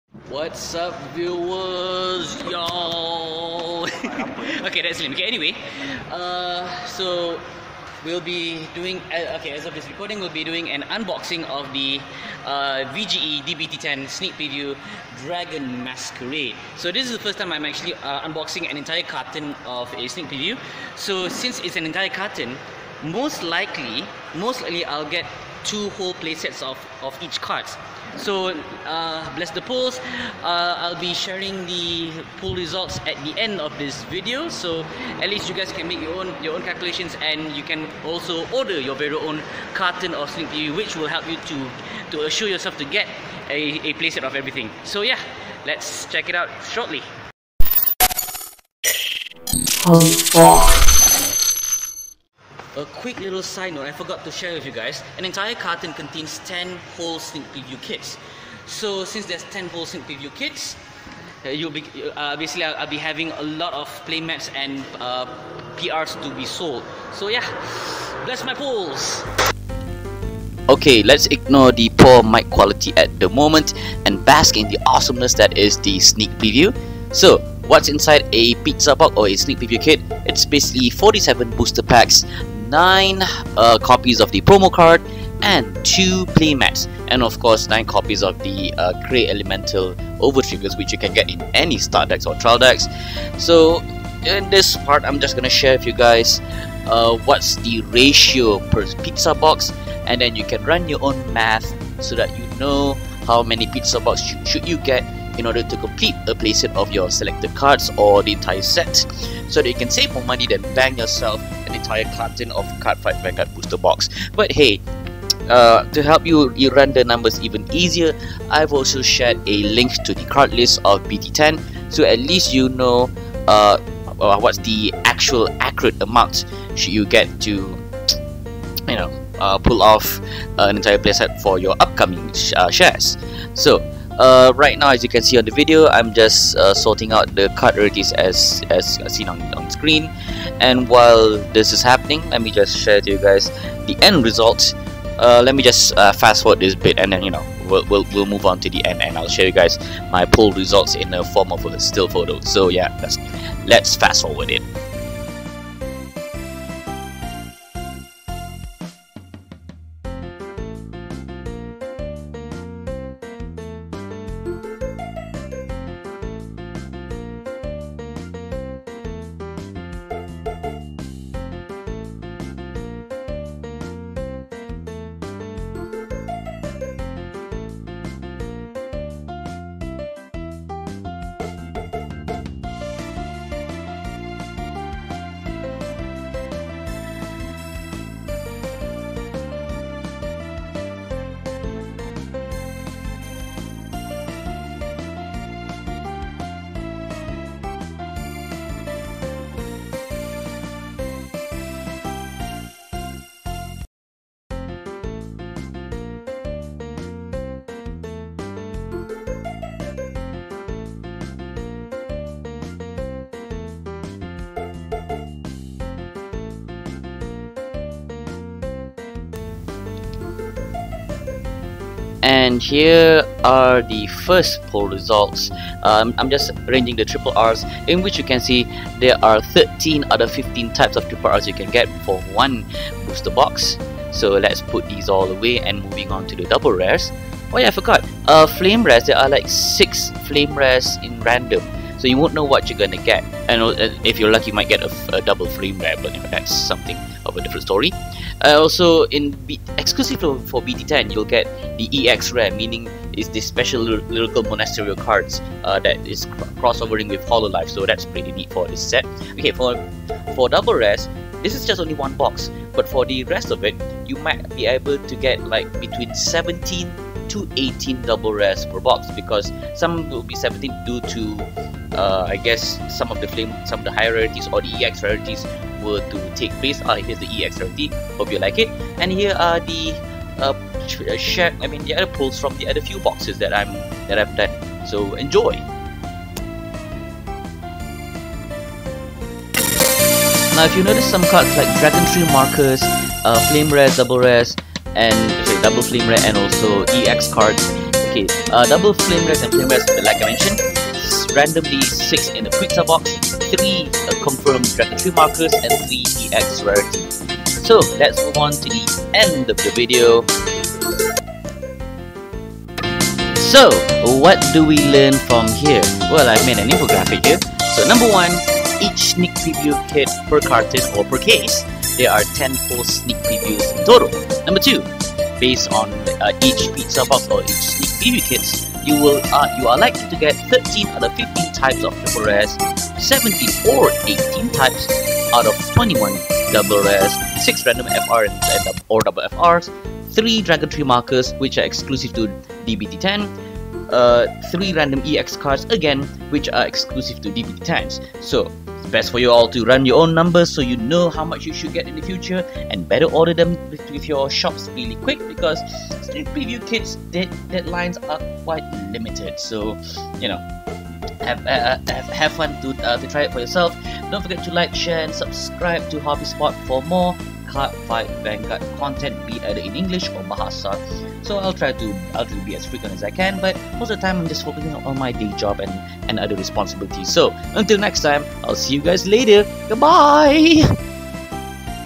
Cardfight!! Vanguard English OverDress BT10 Sneak Preview opening
Here's my opening of the VGE-D-BT10 Sneak Preview kit.

 - Azeem